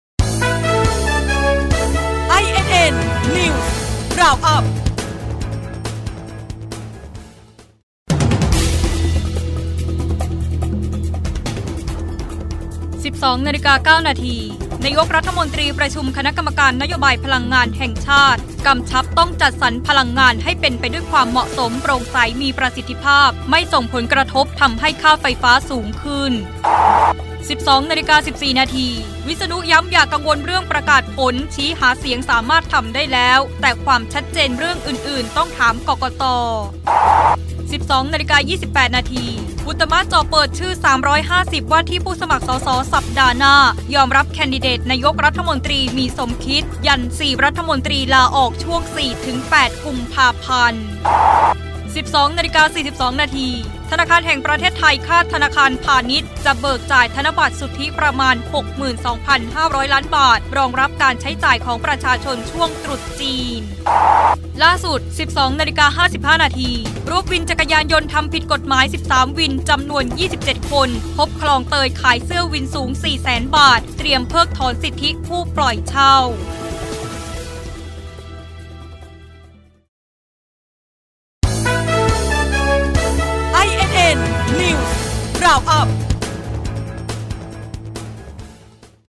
เกาะสถานการณ์ 13.30 น.(24-01-62)
12.09 น. นายกฯประชุม คณะกรรมการนโยบายพลังงานแห่งชาติกำชับต้องจัดสรรพลังงานให้เป็นไปด้วยความเหมาะสม โปร่งใส มีประสิทธิภาพ ไม่ส่งผลกระทบทำให้ค่าไฟฟ้าสูงขึ้น

12.14 น. "วิษณุ" ย้ำ อย่ากังวลเรื่องประกาศผล ชี้หาเสียงสามารถทำได้แล้ว แต่ความชัดเจนเรื่องอื่นๆ ต้องถามกกต.

12.28 น. "อุตตม"จ่อเปิดชื่อ 350 ว่าที่ผู้สมัครส.ส. สัปดาห์หน้า ยอมรับแคนดิเดตนายกรัฐมนตรี มี "สมคิด"ยัน 4 รมต.ลาออก ช่วง 4-8 ก.พ.

12.42 น. ธนาคาร ธนาคารแห่งประเทศไทย  คาด ธนาคารพาณิชย์จะเบิกจ่ายธนบัตรสุทธิประมาณ 62,500 ล้านบาท รองรับการใช้จ่ายของประชาชนช่วงตรุษจีน

12.55 น. รวบ วินจยย.ทำผิดกฎหมาย 13 วิน จำนวน 27 คน พบคลองเตยขายเสื้อวินสูง 400,000 บาท เตรียมเพิกถอนสิทธิผู้ปล่อยเช่า